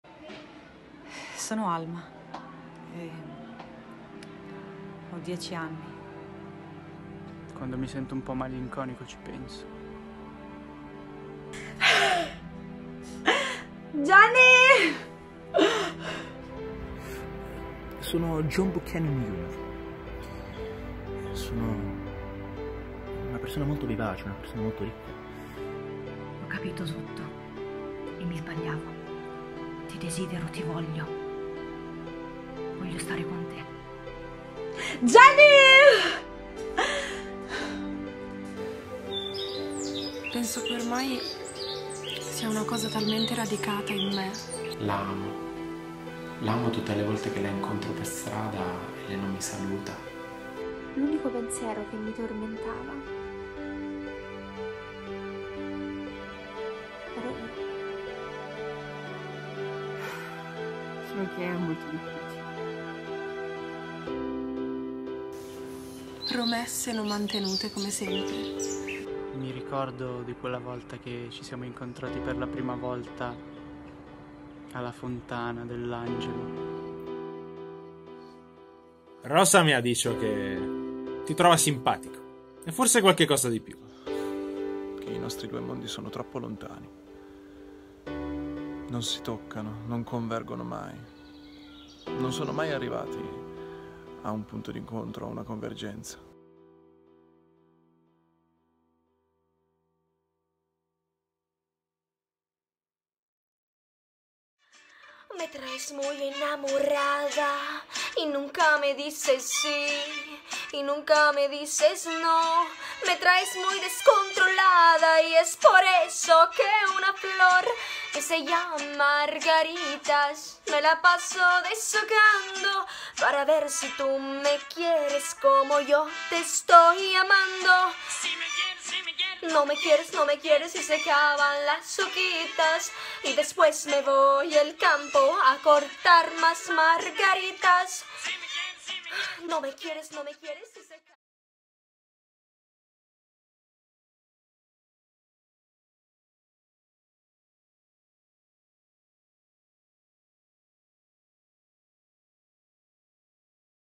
TRAILER ESTATE E FUMO / SECONDO ANNO CORSO ATTORI 2019/2020
Trailer del lavoro finale degli allievi attori del Secondo Anno Corso Attori della Scuola Grock condotto da Pietro De Pascalis.